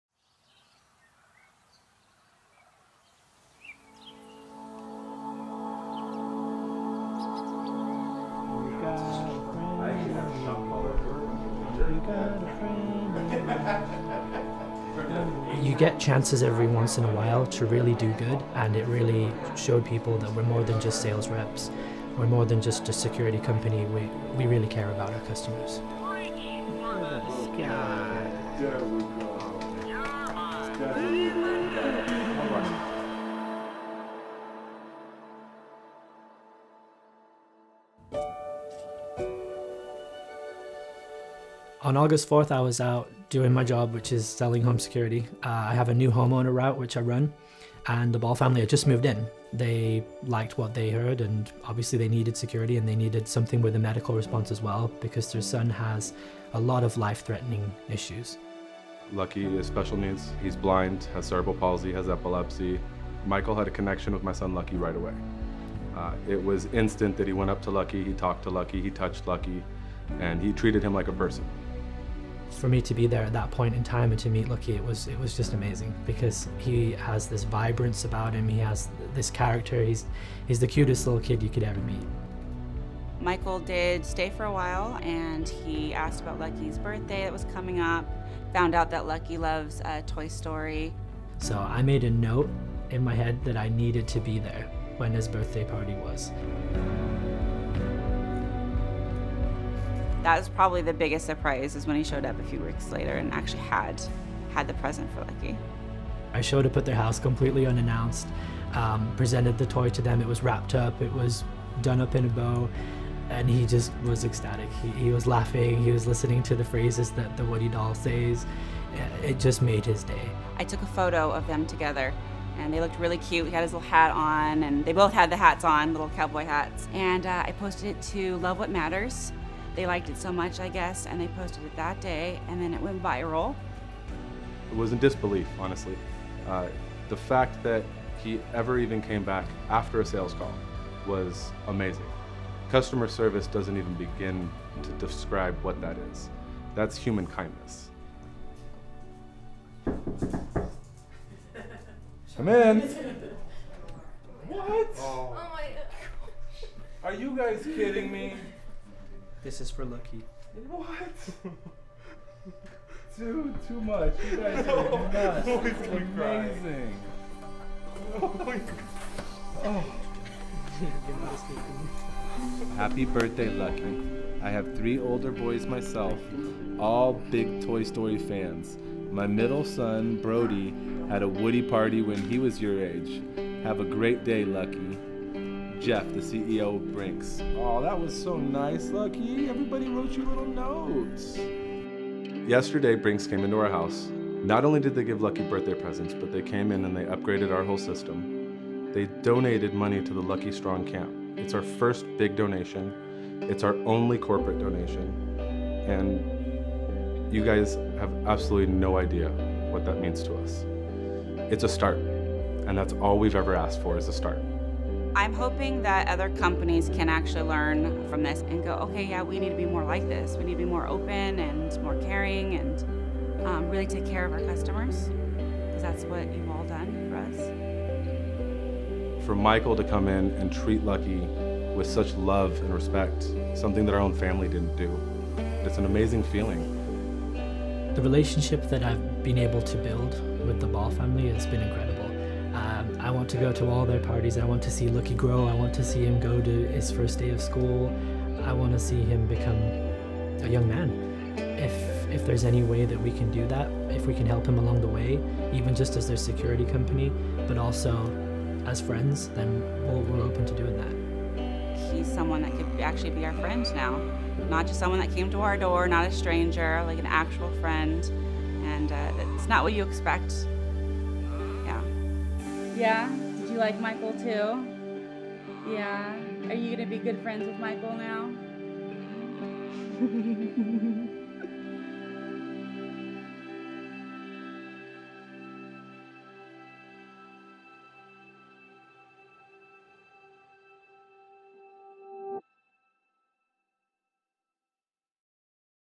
A Lucky Bond
Our customers are why we do what we do! When Michael Moore, a Brinks Home partner with Synergy Security, met Lucky, they didn’t know their new friendship would change lives forever. We’re grateful to be part of stories like this one, and we work hard to make sure that our customers know they’re the reason we do what we do.